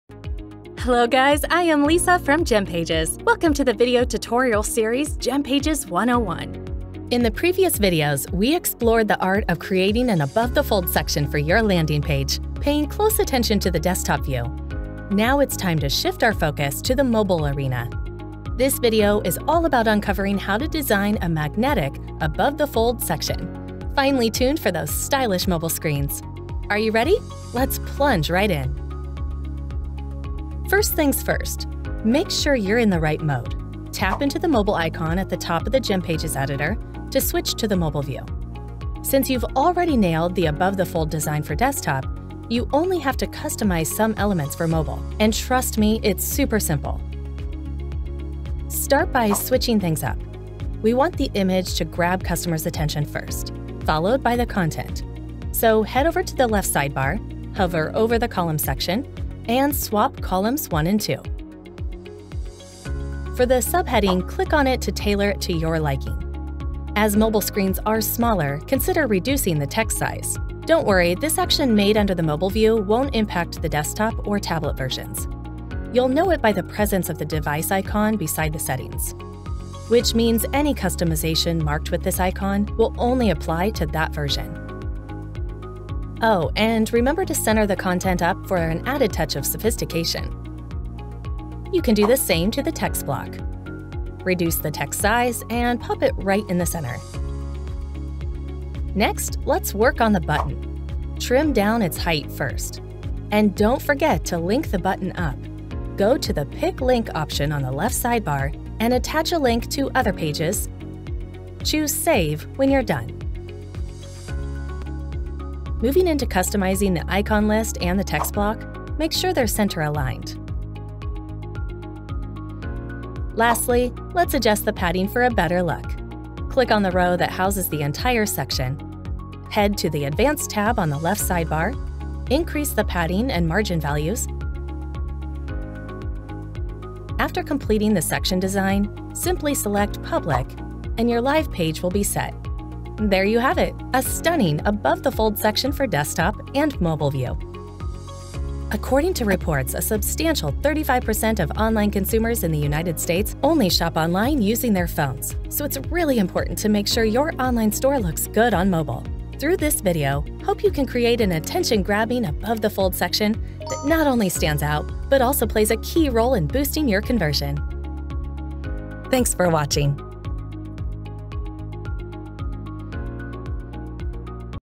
How to Create an Above-the-Fold Section for Mobile View | v7 Editor | GemPages 101
✨ Welcome to GemPages 101 - A tutorial series on using GemPages v7 to build, customize, and optimize your Shopify store with less code. ✨

In today’s video, we’ll show you how to create an effective above-the-fold section for mobile view using GemPages v7.
You’ll learn how to reorder content, adjust headings, style buttons, format icon lists, and fine-tune padding to ensure your mobile landing page looks clean, clear, and high-converting.

🌟 TIMESTAMP:
0:00 - Opening
0:30 - Switch to the mobile view
0:50 - Swap the positions of the image and the content.
1:03 - Customize the heading and sub-heading
1:38 - Customize the button
1:55 - Customize the icon list and the text block
2:02 - Increase the section's padding
2:28 - Closing

🧑‍🎓 𝗖𝗿𝗮𝘃𝗶𝗻𝗴 𝗺𝗼𝗿𝗲 𝗽𝗿𝗼 𝘁𝗶𝗽𝘀? 𝗝𝗼𝗶𝗻 𝗚𝗲𝗺𝗣𝗮𝗴𝗲𝘀 𝗔𝗰𝗮𝗱𝗲𝗺𝘆